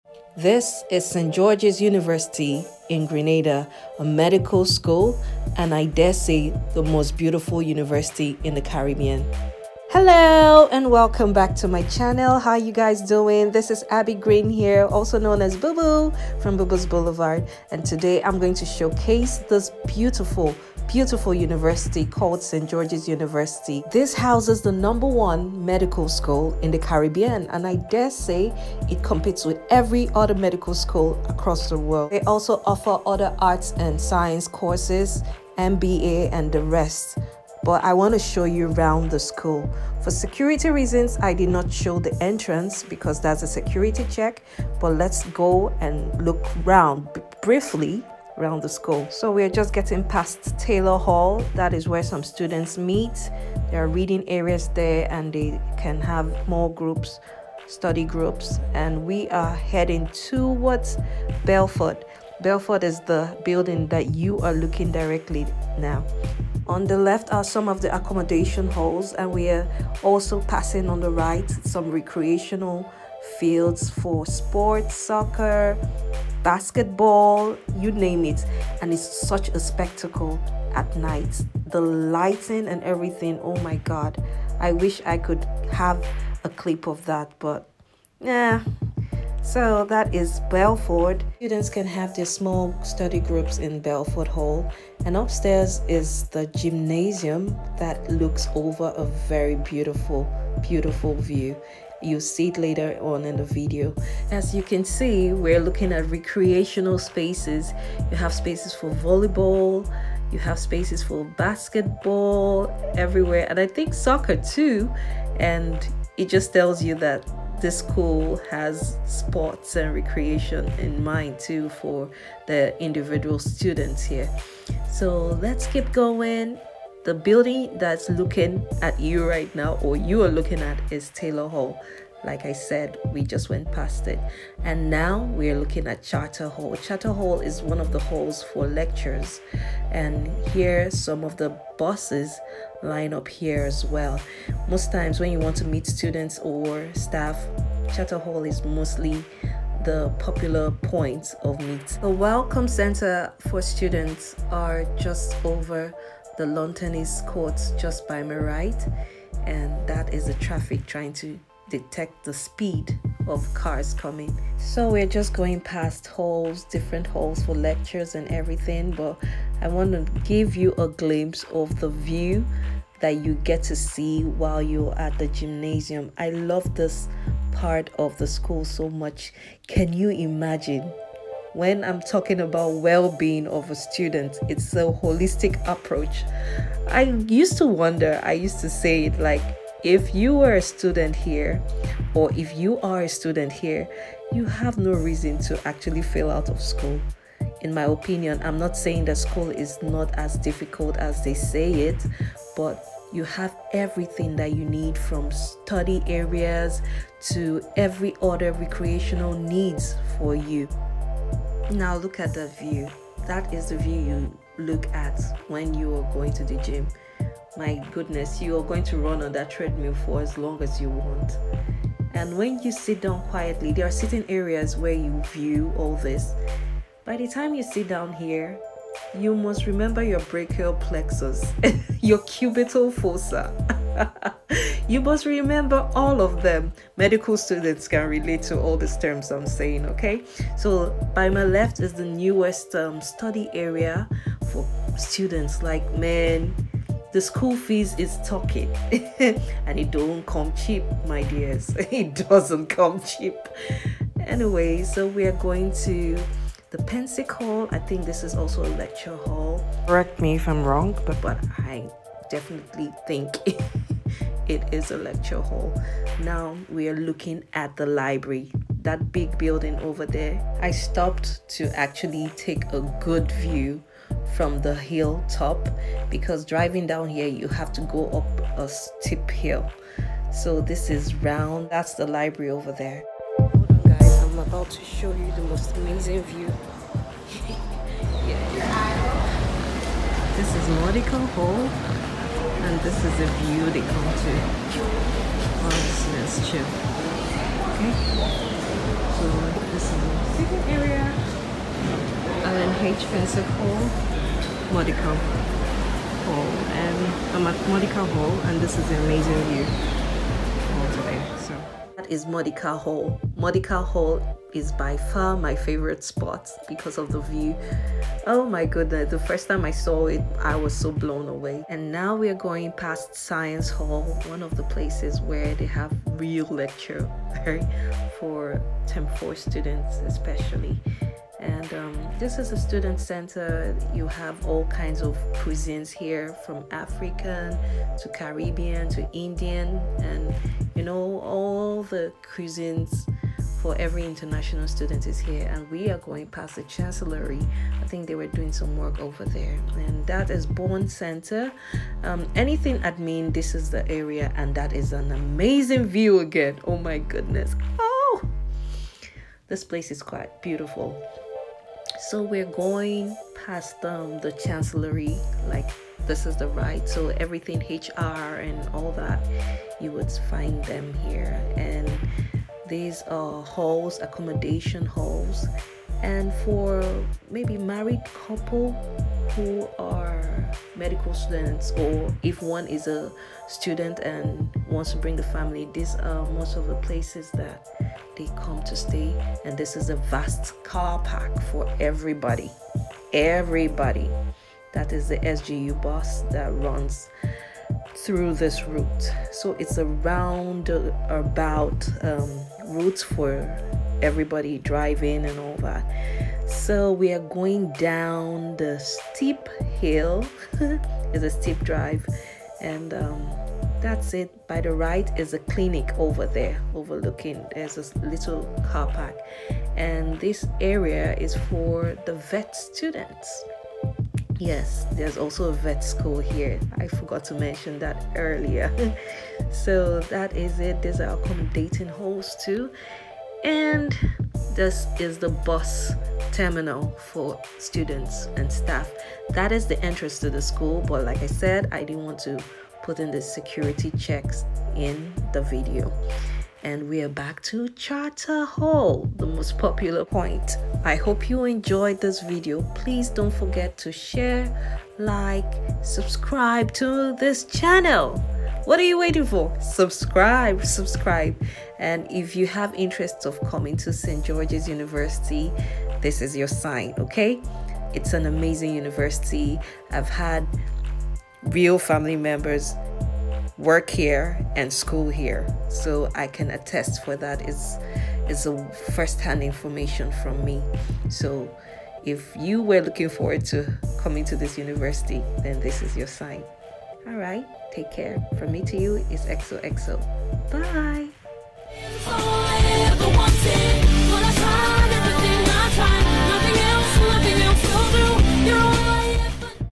The Most Beautiful Medical University In The Caribbean| St George’s University
This has been a long-awaited video for all the right reasons. Good things come to those who wait. In my opinion St George’s has it all; resources for Students and Staff both for Academia and leisure. Today, I take you on a quick tour of this beauty. I’d leave the nitty-gritty for an official orientation from the school but this video is just good enough. So, if you had eyes for an excellent Medical school or any other Art, Science and social science courses to study, here’s your plug. If you contemplated a Medical School, here’s your sign! This gem is in True Blue, Grenada.